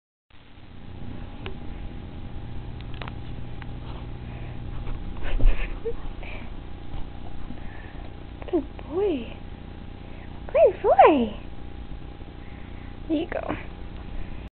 The Hobbes trick!
hobbes the corgi performing his special trick, called "up"!!!